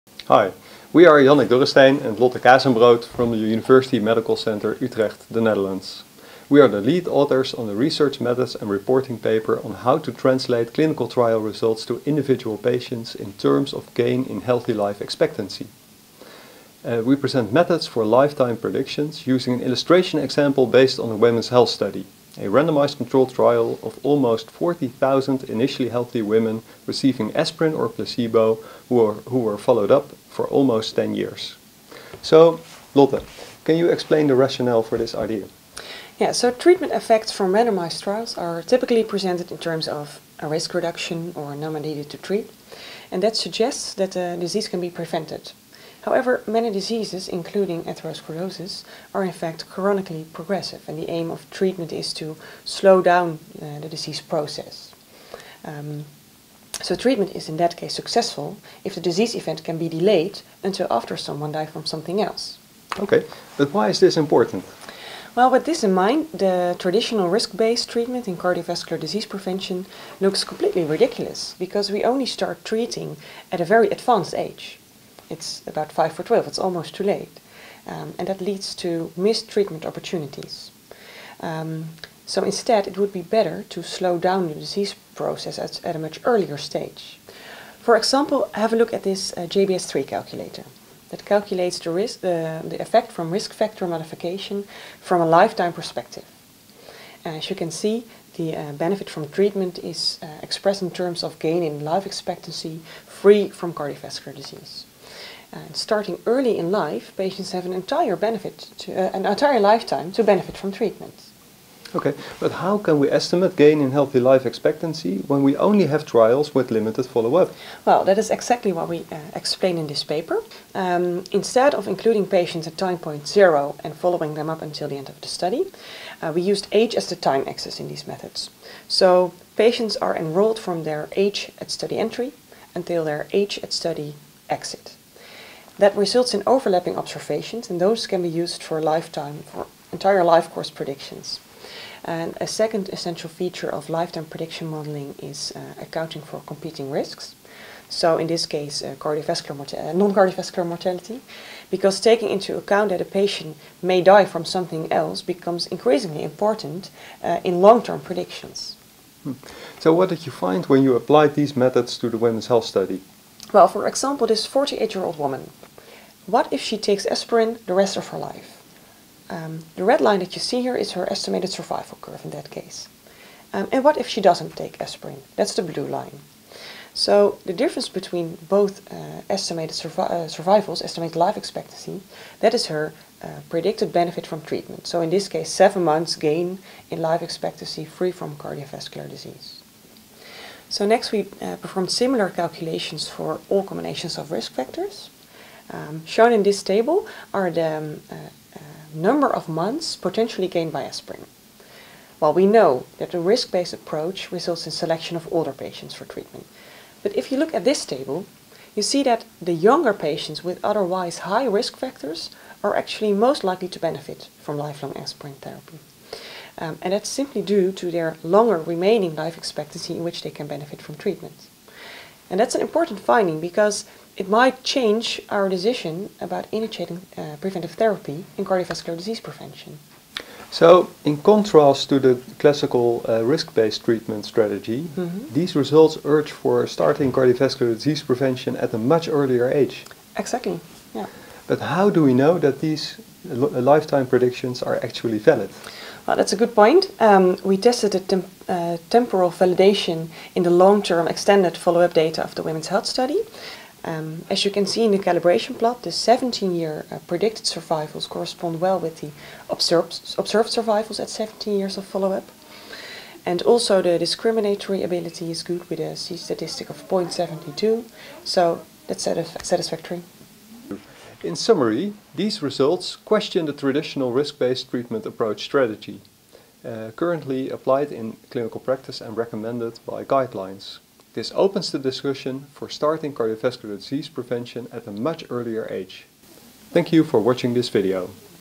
How to translate clinical trial results into gain in healthy life expectancy for individual patients
Treatment effects from randomised trials are typically expressed as numbers needed to treat to prevent one adverse disease event during a fixed time interval (eg, five or 10 years).

In the actual patient, however, many diseases are chronically progressive, despite treatment, e.g. diabetic nephropathy, some types of malignancies, osteoporosis, and atherosclerosis.

In these examples, the aim of treatment is not to prevent but to delay the occurrence of symptomatic disease. Thus the actual effect of treatment is gain in disease-free life expectancy.

Healthy life expectancy is a customary outcome type in cost effectiveness studies at group level, but predictions can also be made at individual patient level.

In this article the authors explain how clinical trial results can be translated into gains in healthy life expectancy for individual patients. We use aspirin in the primary prevention of cardiovascular disease as an illustrative example, based on real data from the Women’s Health Study.